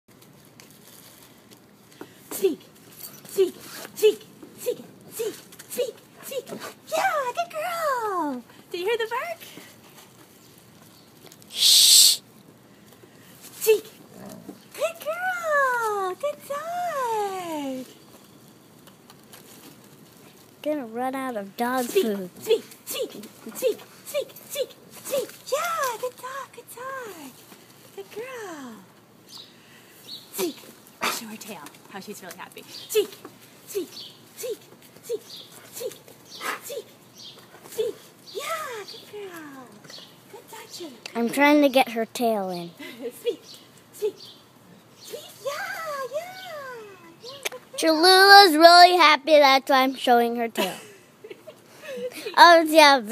Cholula huffs
Cholula tries to bark on command